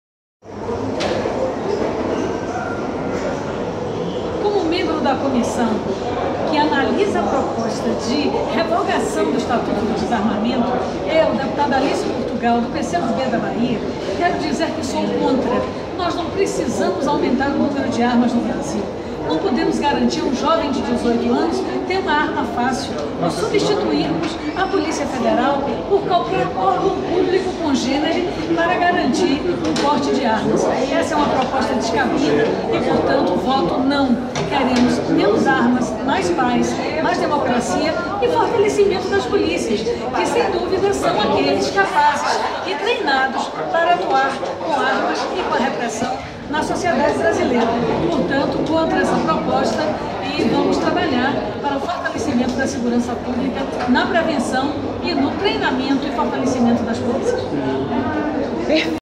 Deputada Alice Portugal do PCdoB/BA é contra o armamento.
A deputada é membro da comissão da especial que analisa a proposta de revogação do Estatuto do Desarmamento, o PL 3722/12. Ela quer mais paz e menos armas na sociedade. Veja o que ela diz!